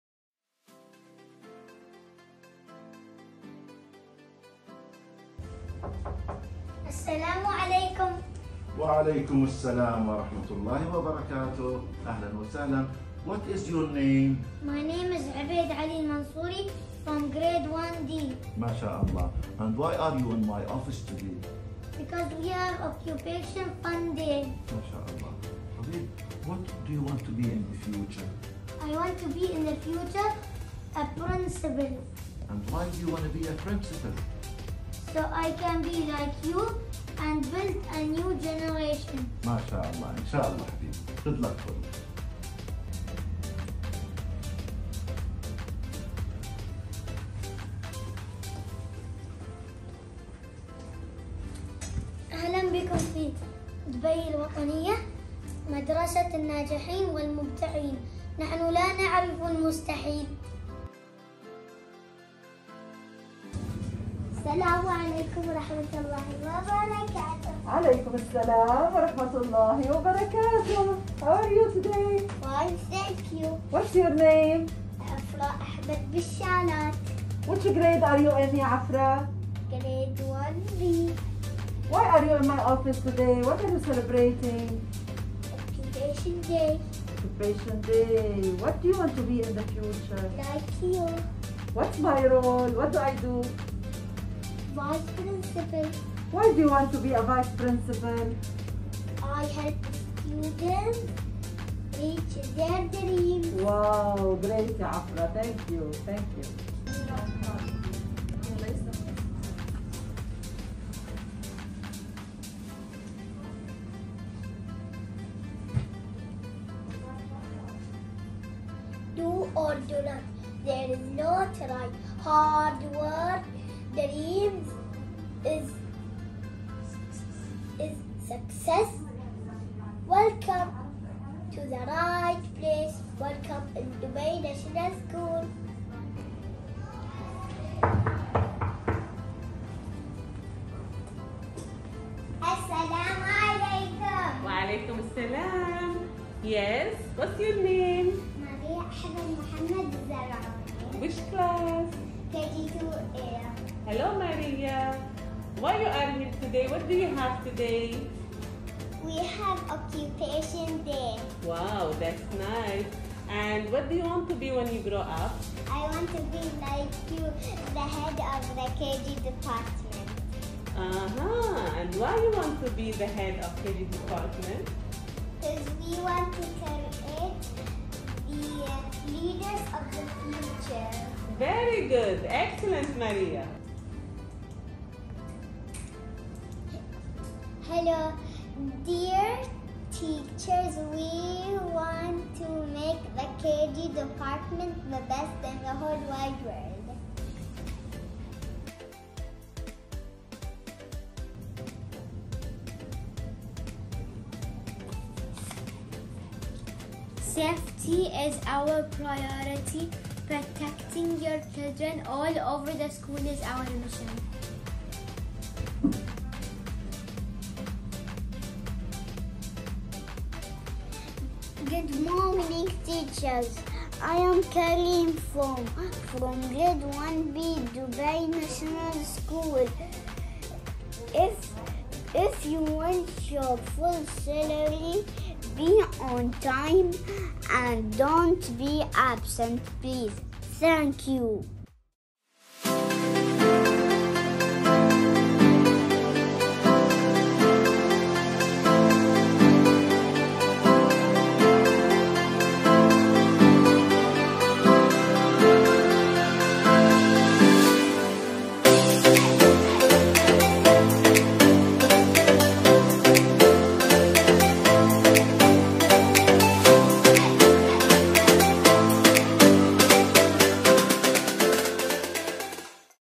June 6, 2021
⬅️خبر عاجل✴️
تم الاستيلاء على بعض المناصب الإدارية في المدرسة من قبل مجموعة من 🧑🏽👦🏼 المبدعين الصغار 👧🏻👧🏼🧒🏽وذلك لإثبات طموحاتهم والبدء برسم مستقبلهم. وقد اتخذت الإدارة الإجراءات المناسبة بتشجيعهم وإفساح كل المجالات لهم للتحليق في عالم الإبداع والمعرفة والتصورات اللانهائية.